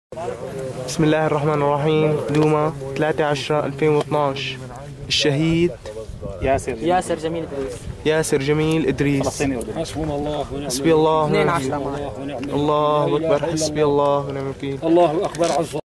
Syria | Douma | Yasser Jameel Edreess | October 03, 2012 | 18+ ONLY
Yasser Jameel Edreess was extra judicially executed by Al-Assad's militias
in Douma City on October 02, 2012